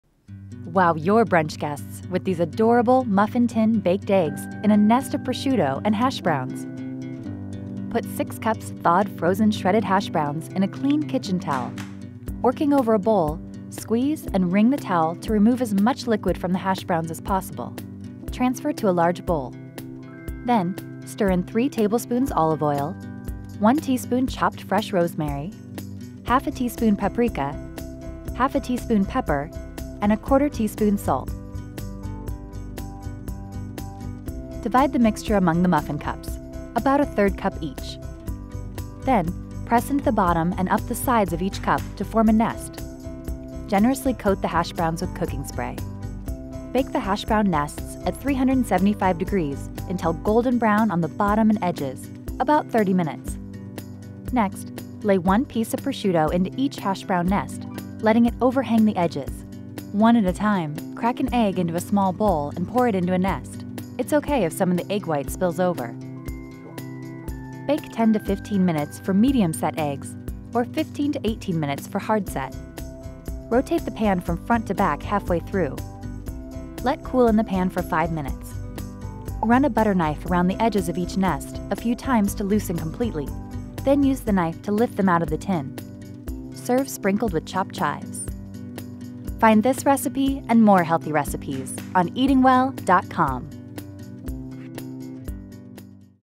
How to Make Baked Eggs in Your Muffin Tin
Wow your brunch guests with these adorable muffin-tin baked eggs in a "nest" of prosciutto and hash browns. Go beyond standard breakfast fare with this perfectly-portioned baked eggs recipe that's both healthy and delicious.